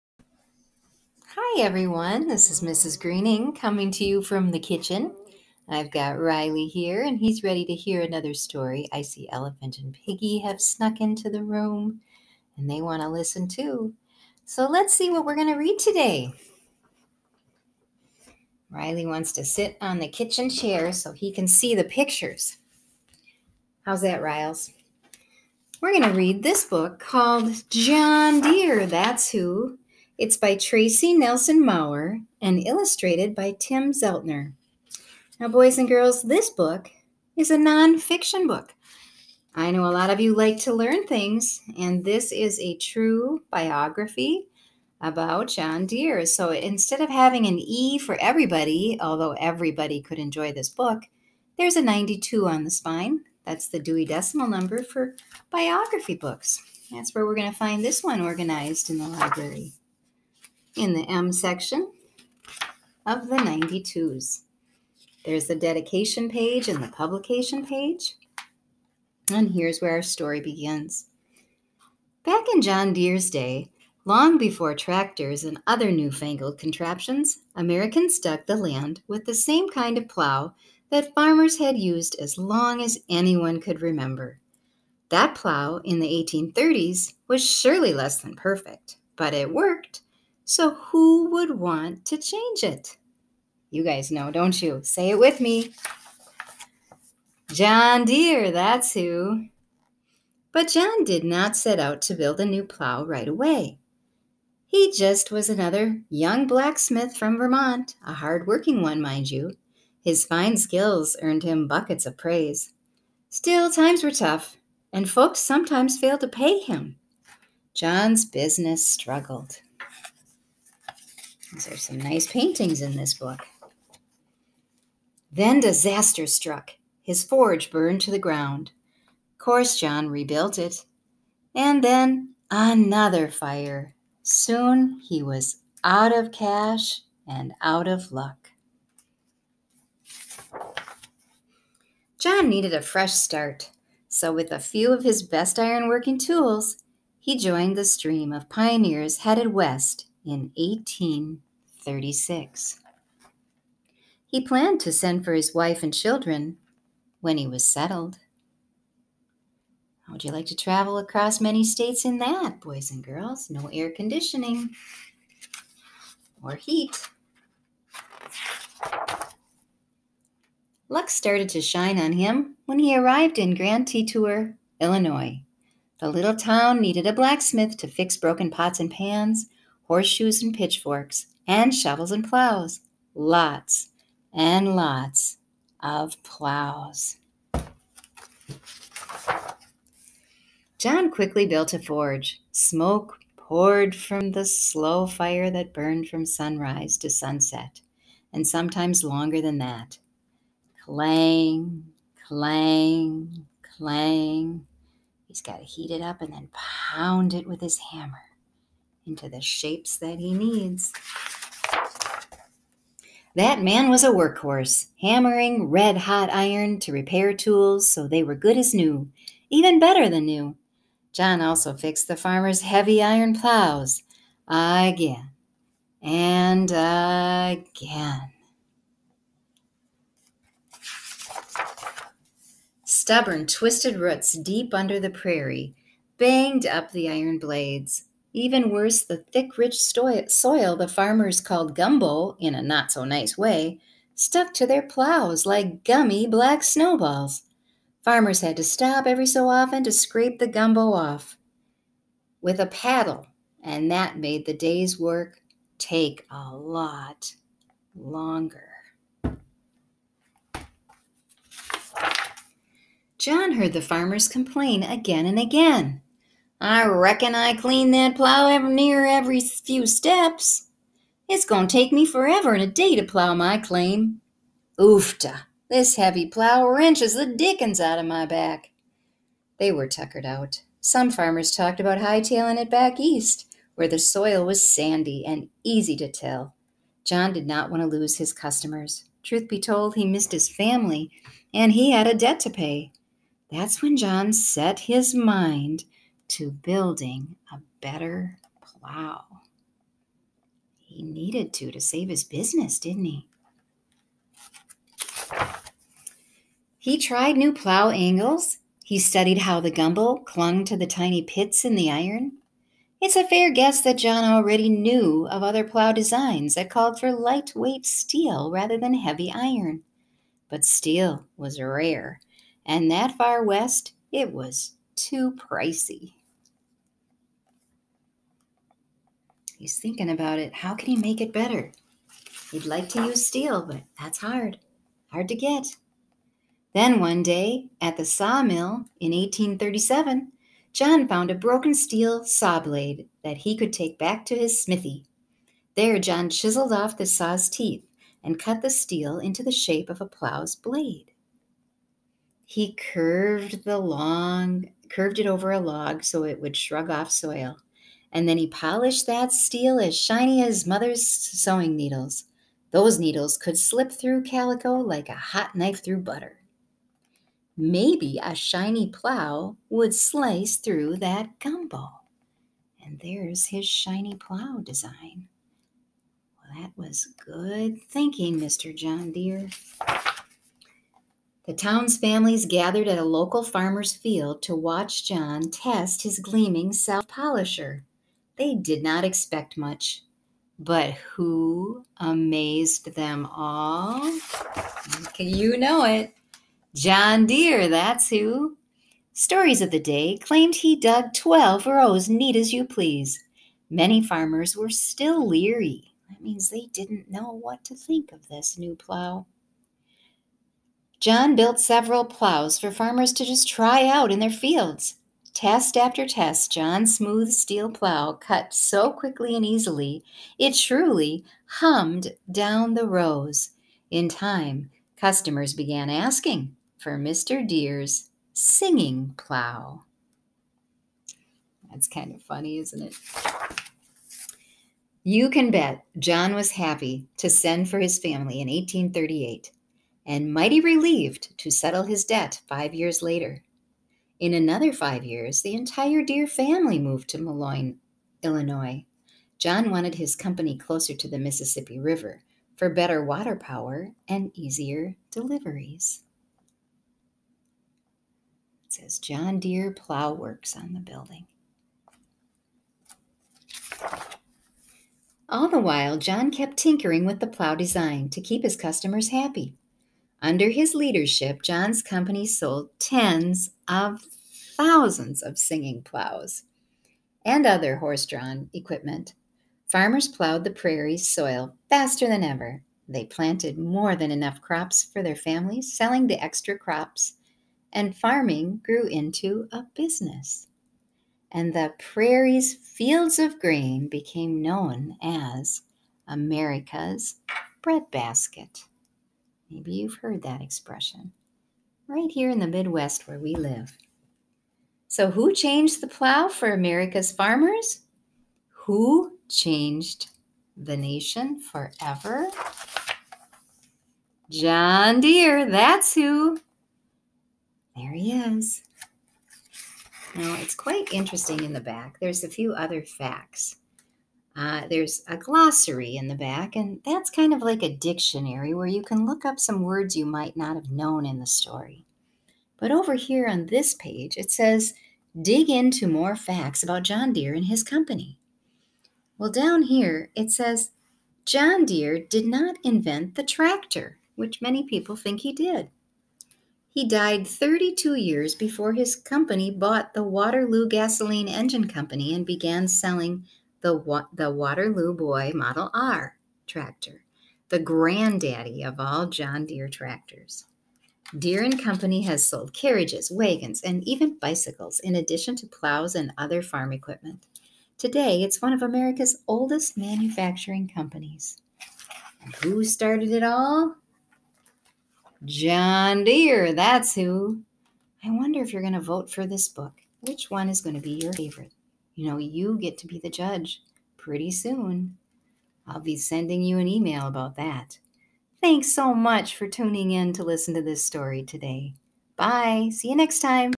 Star of the North book:John Deere, That's Who! by Tracy Nelson Maurer; Illustrated by, Tim Zeltner
Open this book with me to learn about inventor John Deere.
Way back in the 1830's, who was a young blacksmith from Vermont, about to make his mark on American history?
John Deere, that's who!